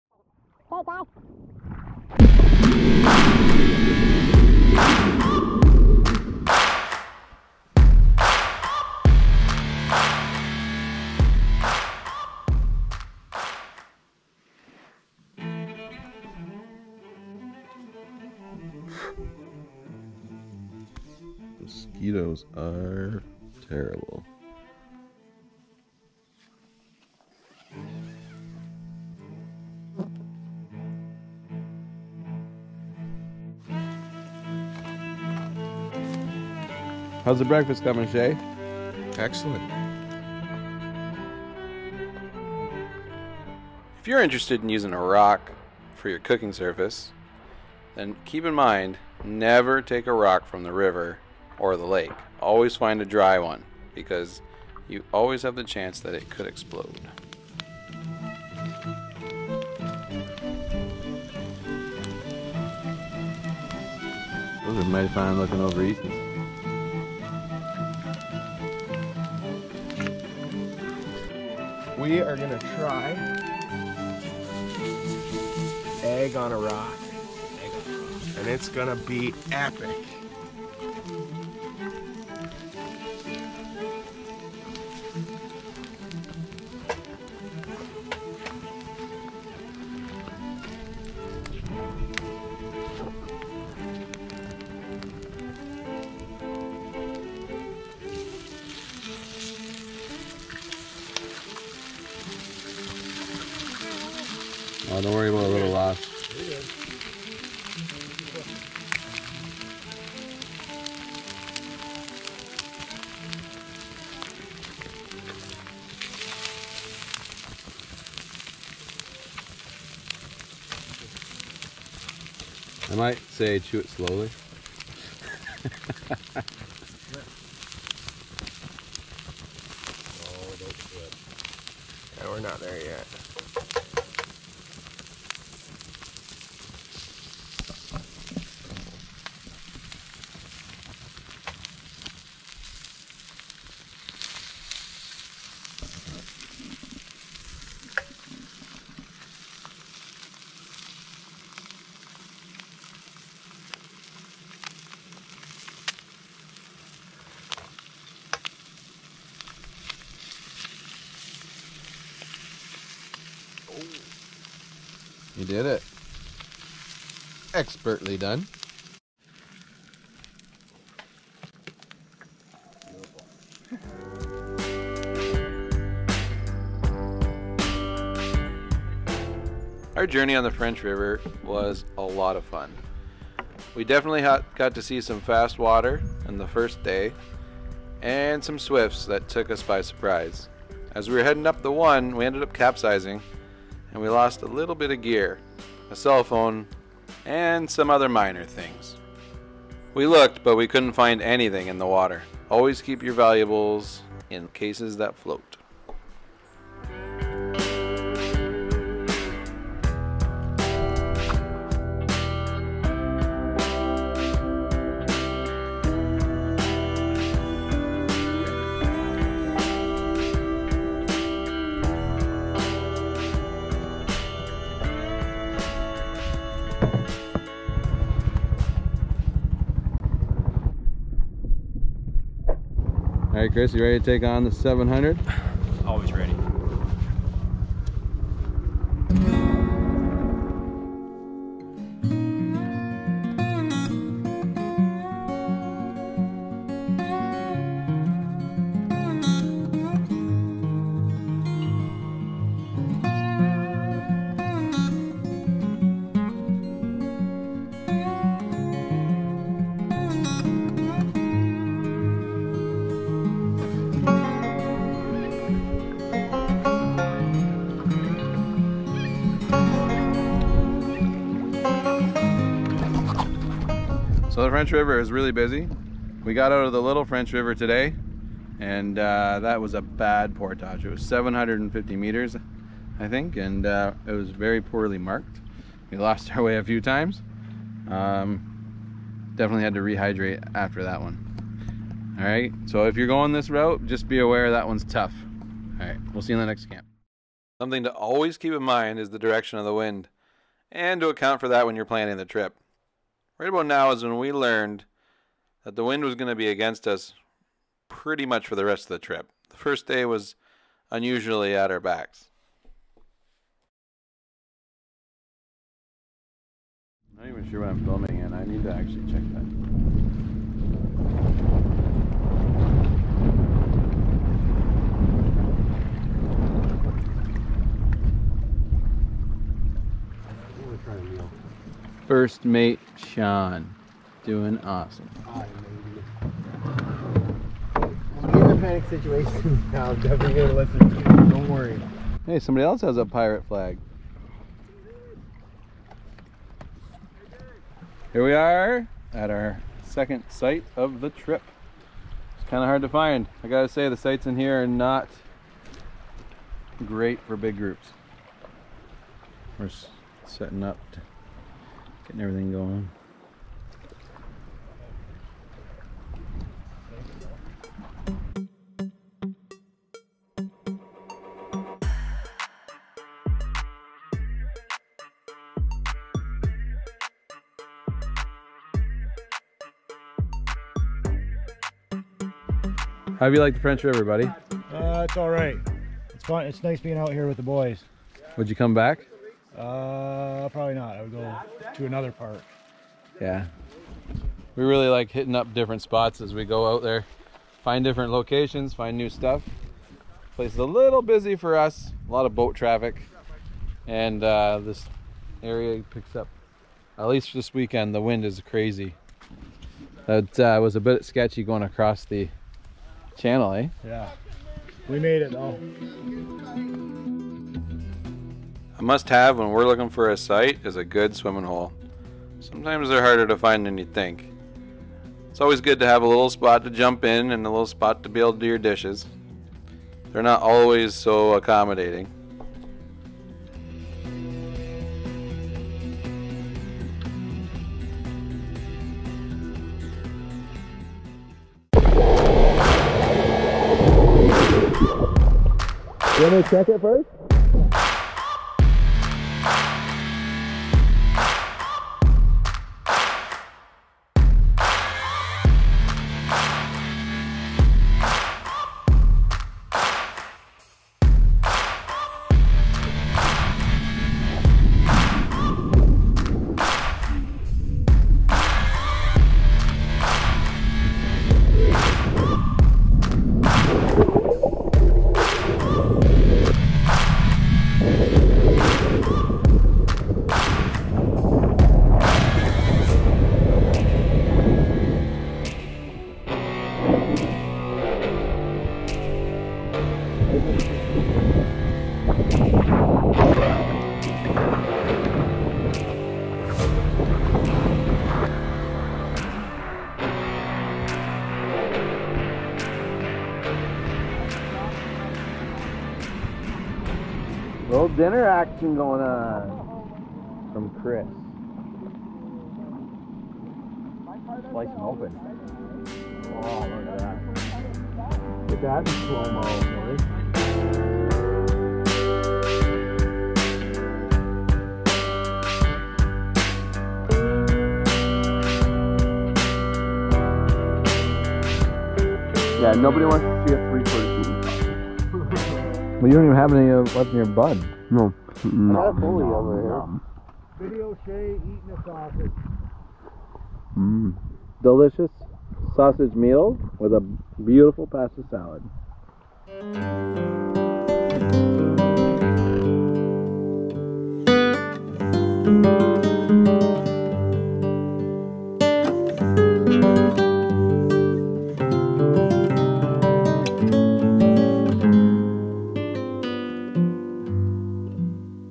Day 2, A Backcountry Canoeing Adventure on the French River
A great adventure down a historic river in Canada.  The French River was a major trade route in the past.  Things as basic goods and services were traded for furs.  We enjoyed exploring the upper section of the river near Lake Nipissing.
In this video we travel a long stretch of the river, much of which was against the wind, and share with you our camp life.  I hope you enjoy it, and that it may inspire you to get out and enjoy this activity with your friends.